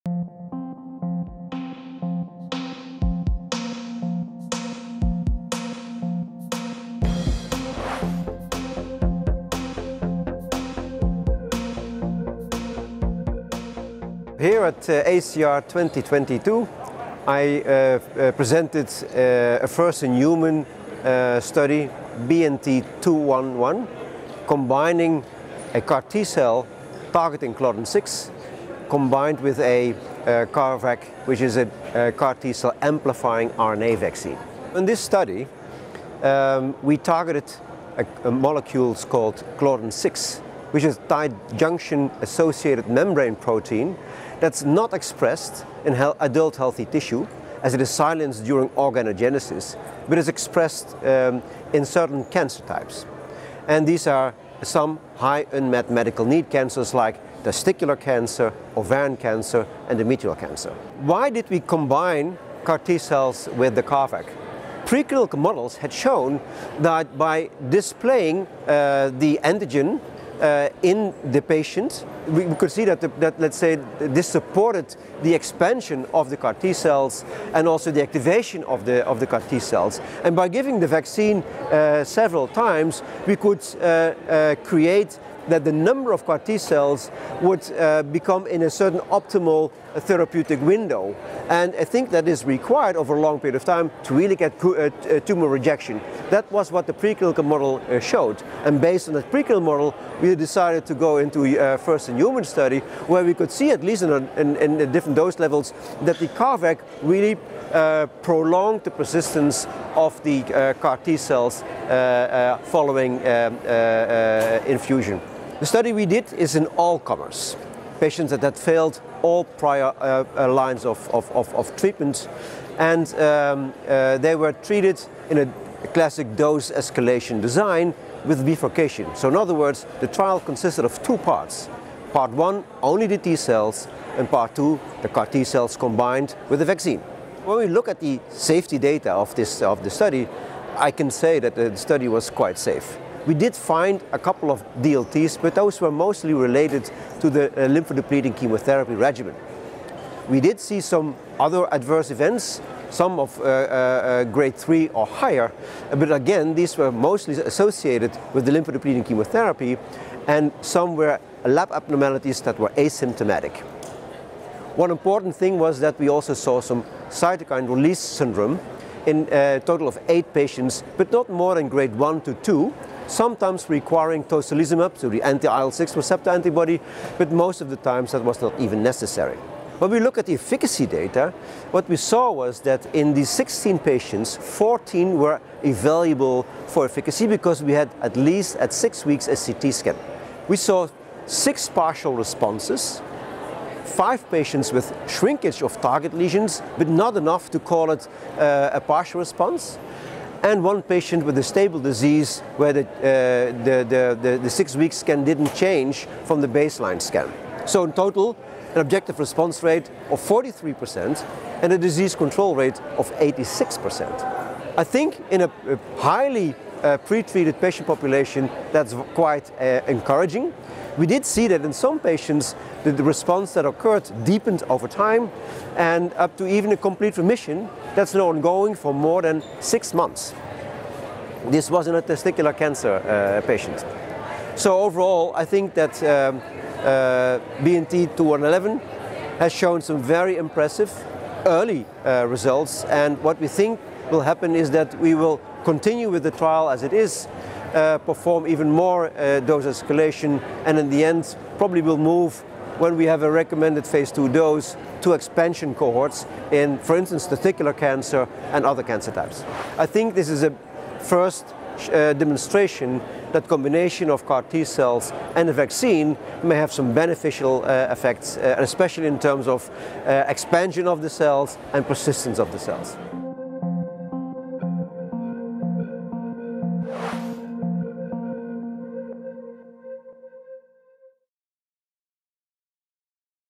Highlights on CAR T cells in advanced solid tumours @AACR 2022
Reporting onsite from AACR 2022 in New Orleans, John Haanen explains the results from a phase I study testing CAR T cells targeting Claudin-6 which is a protein not expressed in adult healthy tissue, but in certain ovarian, endometrial and testicular cancers. The study was carried out in patients who had previously failed all treatments. Combined with a CAR T cell amplifying RNA vaccine to prolong the CAR T cells persistence, this novel therapy showed promising early results in terms of response and safety.
Reference :
Haanen J, Mackensen A, Koenecke C, et al. BNT211: A Phase I trial to evaluate safety and efficacy of CLDN6 CAR-T cells and CARVac-mediated in vivo expansion in patients with CLDN6-positive advanced solid tumors. AACR Annual Meeting 2022; New Orleans, Louisiana, USA: 8–13 April.